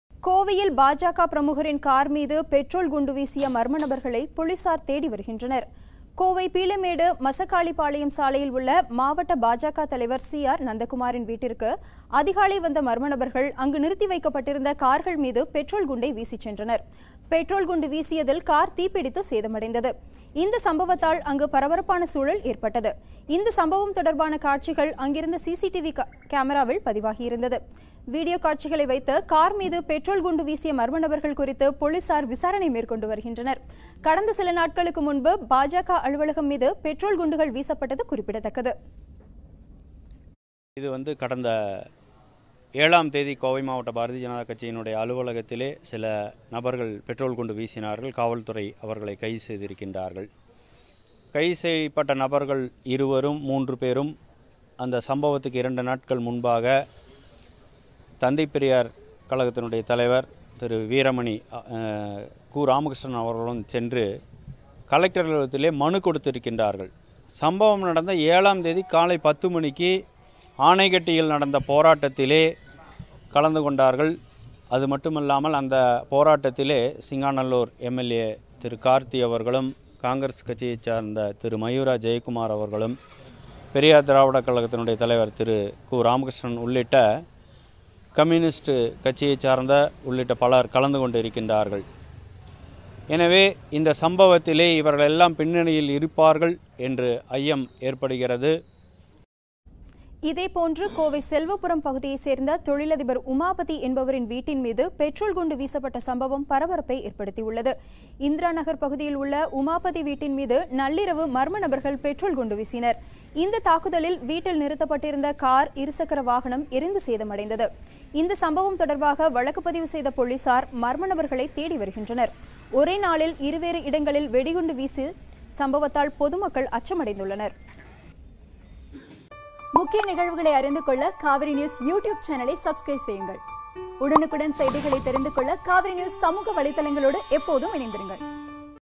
பாஜக பிரமுகரின் கார் மீது பெட்ரோல் குண்டு வீச்சு | Petrol bomb Hurled in BJP functionary's house
கோவை: பாஜக பிரமுகரின் கார் மீது பெட்ரோல் குண்டு வீச்சு | Petrol bomb Hurled in BJP functionary's house